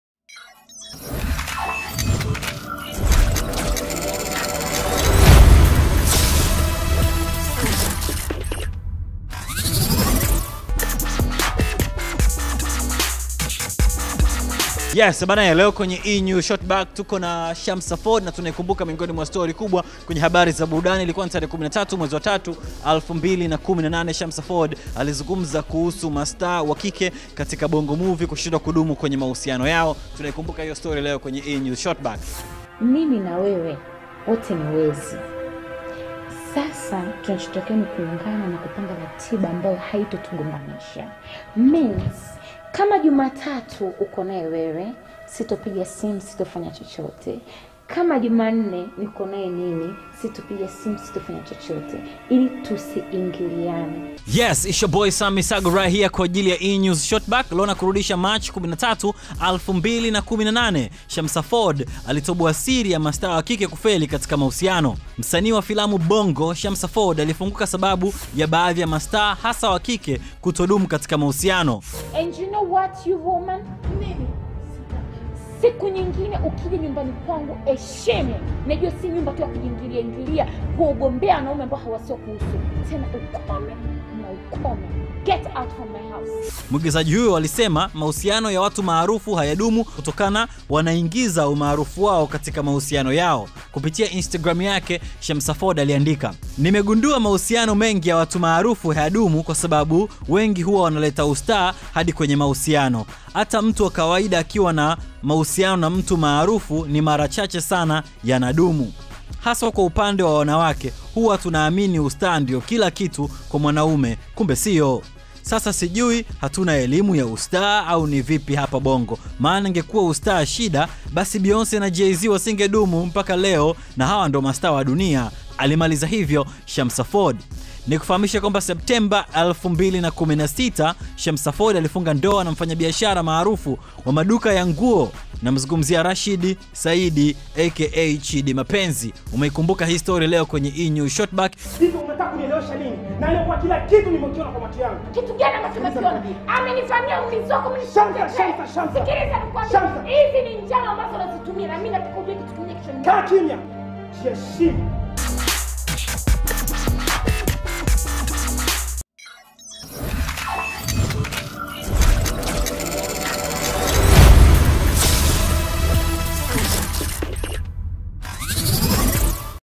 March 13, 2018 Shamsa Ford alitoa siri za mastaa wa kike kufeli katika mahusiano.
Msanii wa Filamu Bongo, Shamsa Ford alifunguka  sababu ya baadhi ya mstaa hasa wa kike kutodumu katika mahusiano.

Muigizaji huyo alisema mahusiano ya watu maarufu hayadumu kutokana wanaingiza umaarufu wao katika mahusiano yao.